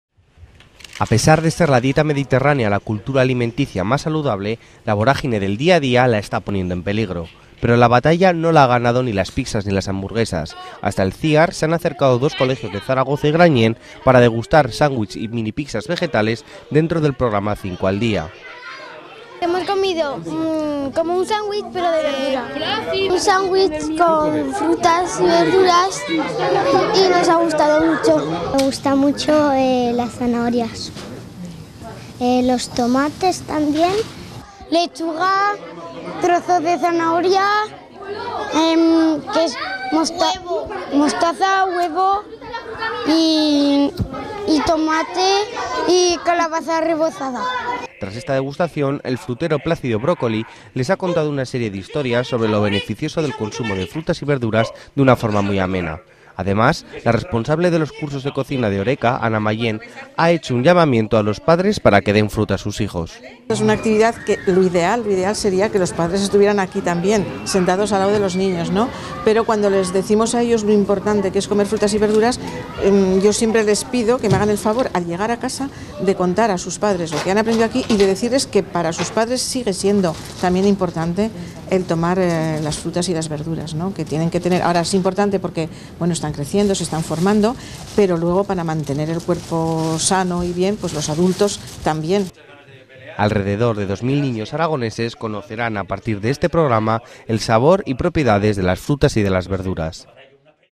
Cinco frutas al día, la batalla de los más pequeños contra la comida rápida
Alrededor de dos mil niños de 40 colegios aragoneses serán los que participen directamente en las actividades de la campaña 5 al día para la promoción del consumo de frutas y verduras entre los más pequeños. Este jueves, dos colegios ya han podido formarse, con un toque de divertido, en lo que se refiere a la dieta mediterránea.